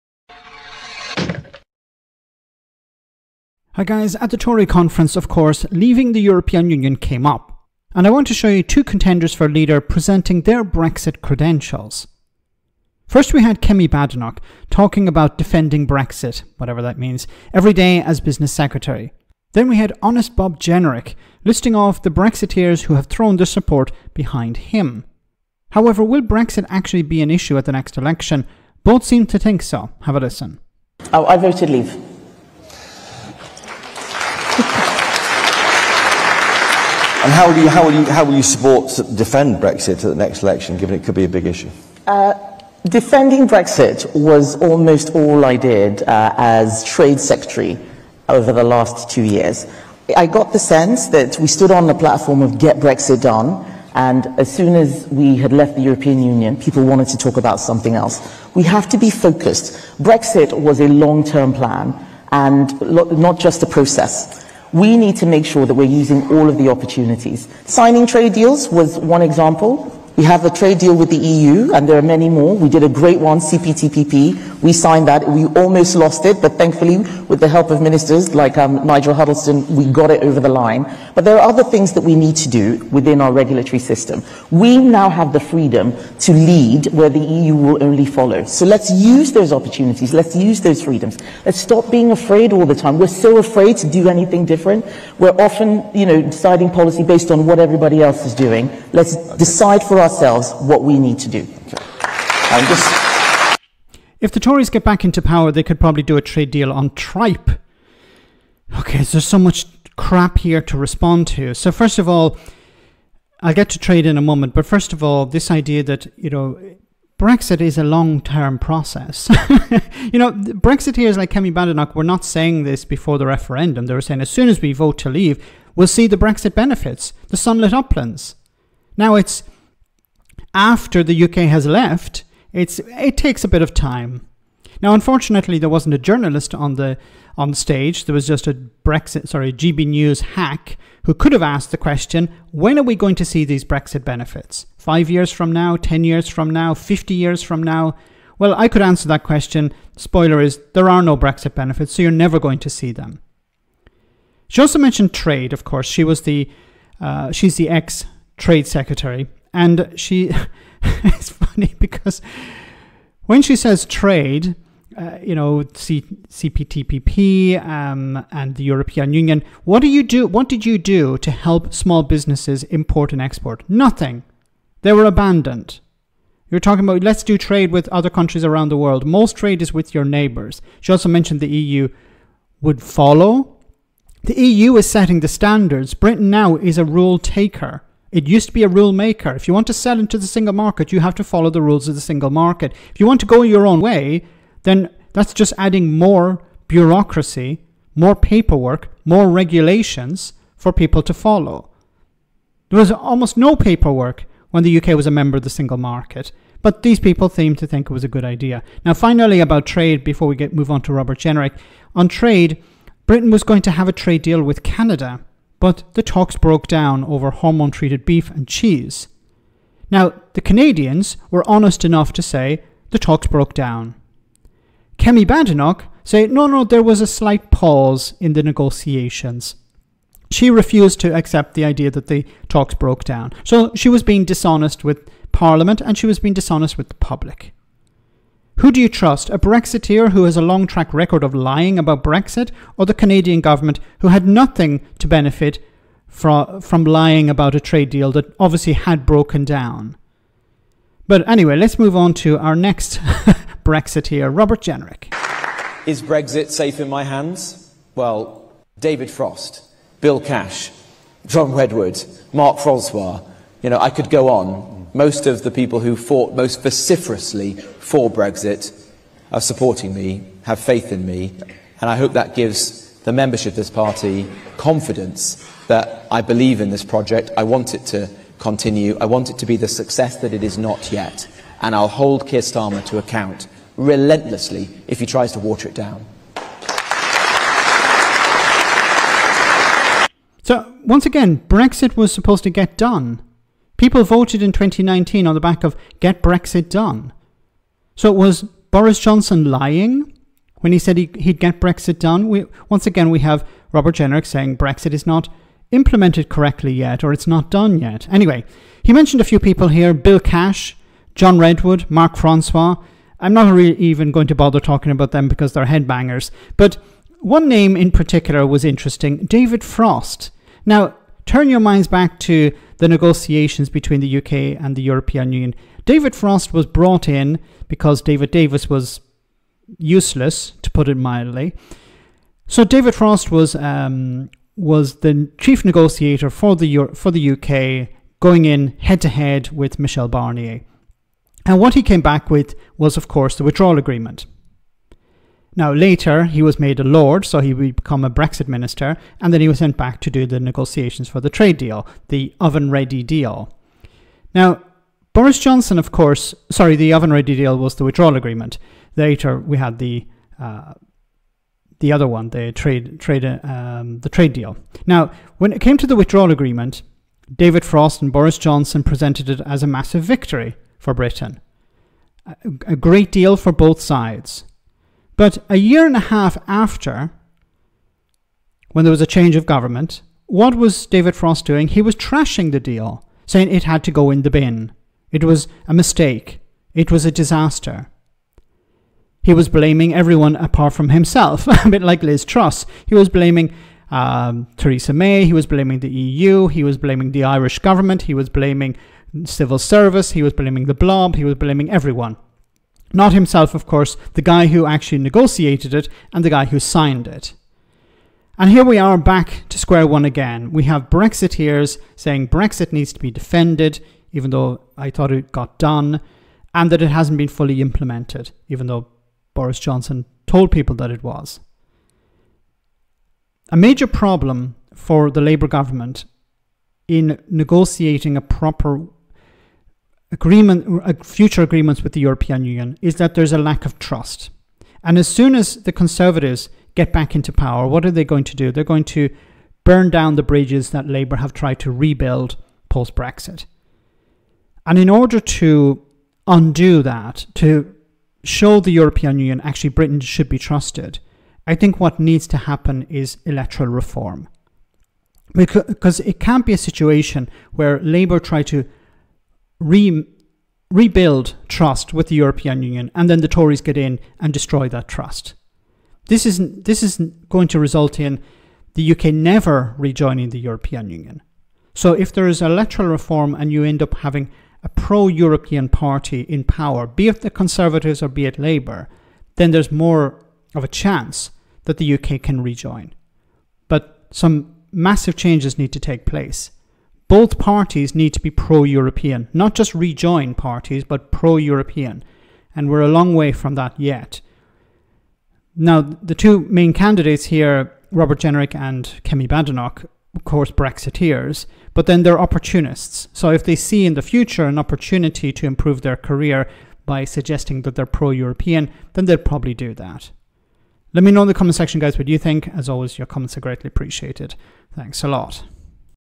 Kemi Badenoch And Robert Jenrick Talk Brexit!
At the Tory party conference, Kemi Badenoch and Robert Jenrick were asked about Brexit and while one doesn't understand trade the other doesn't understand that the guy he held up as a back is the same one who trashed his own deal...

Credit: The Telegraph